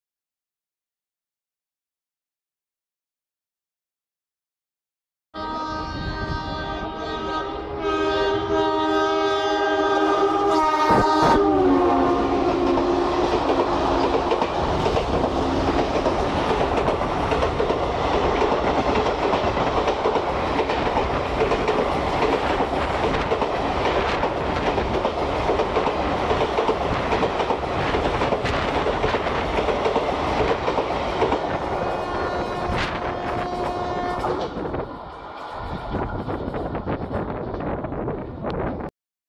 Dadar Ajmer Superfast Skips Saphale with WCAM 1
Location: Saphale
Time: 15:49
Camera: Nokia E7

The Ajmer bound 12989 Dadar - Ajmer Superfast is seen here hauled by a WCAM 1 workhorse while skipping Saphale at a good speed. Video captured from Virar bond Vapi Virar Passenger.